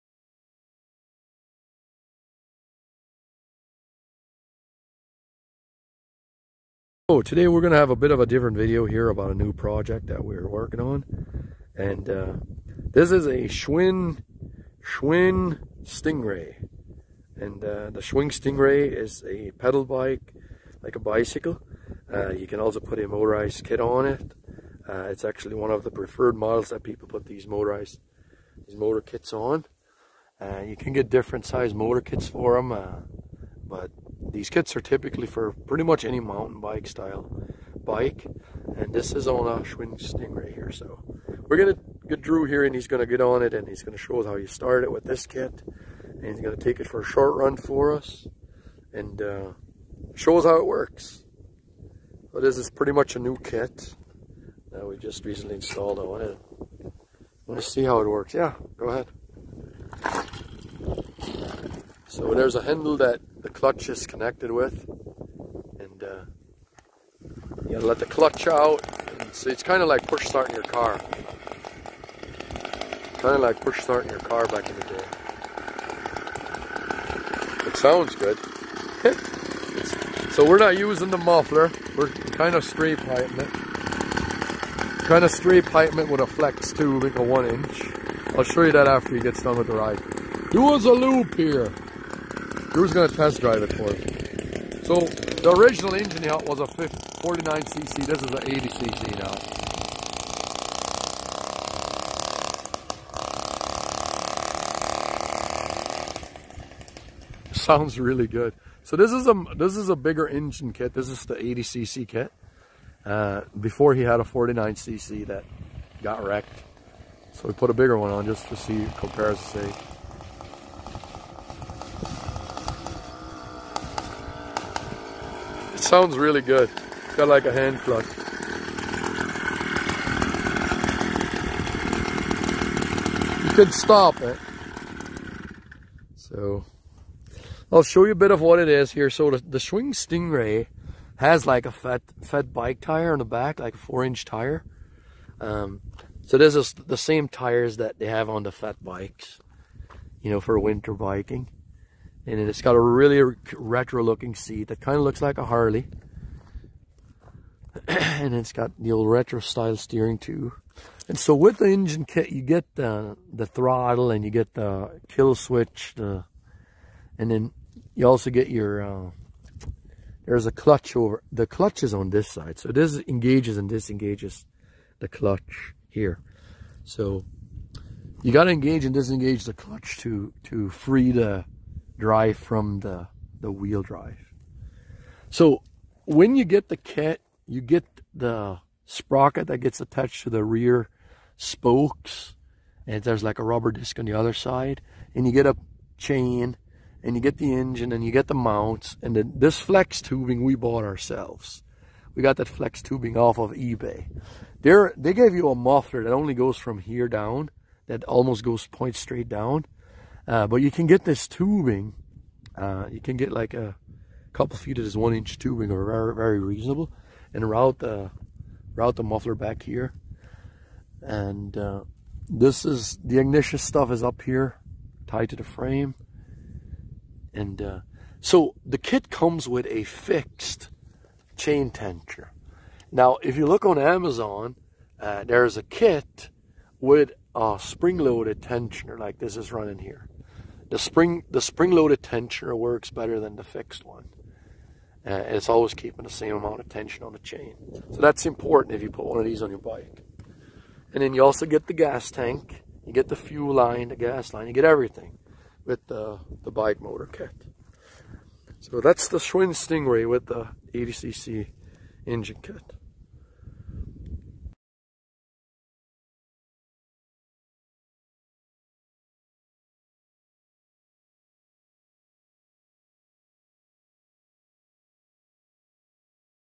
Schwinn Sting Ray Bicycle powered by 80cc 2cycle Bike Motor Kit
Reviewing my sons project; a Schwinn Sting Ray bicycle with a 88cc Bicycle Motor Kit. This kit fits most regular bicycles, including the retro looking Schwinn Sting Ray. I have included the links to the parts we used on this project.